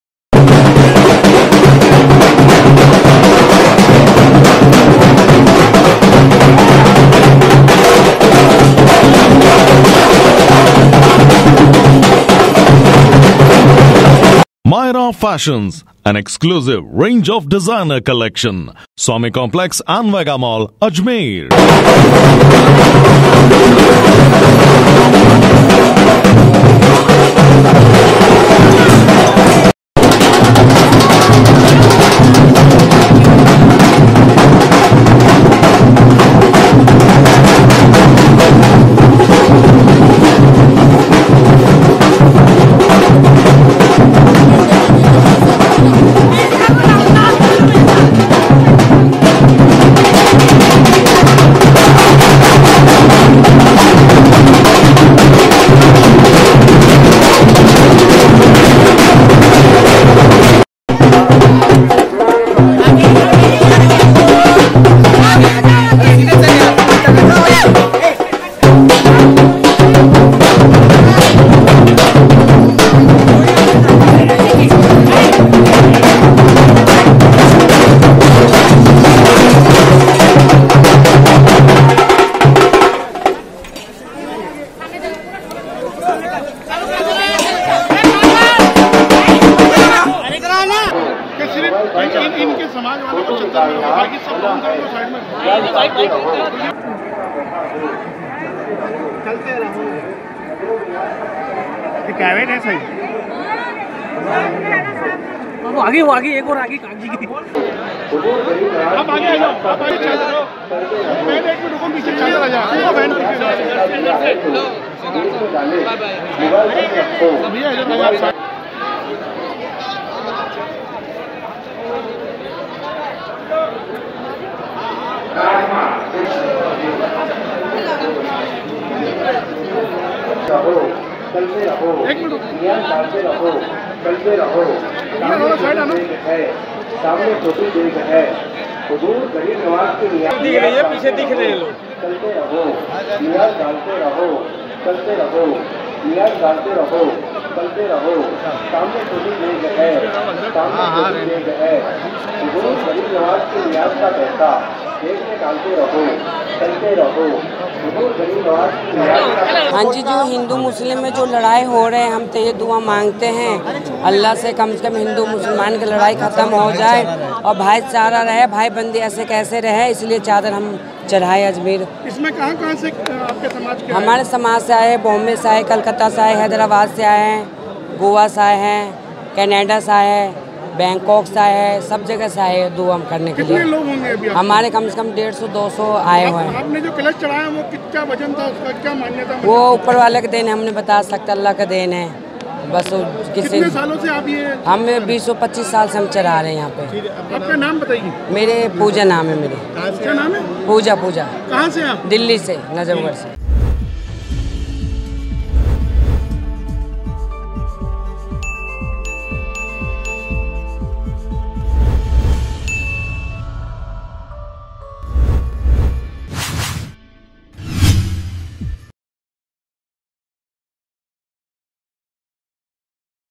AJMER NEWS | ख्वाजा साहब का 808 वां उर्स | MTTV INDIA
ख्वाजा साहब का 808 वां उर्स
किन्नरों ने उर्स के मौके पर चांदी का कलश किया पेश
शुक्रवार को किन्नर धूमधाम से दरगाह में जियारत करने पहुँचे
पाकिस्तानी जत्था भी करेगा ज़ियारत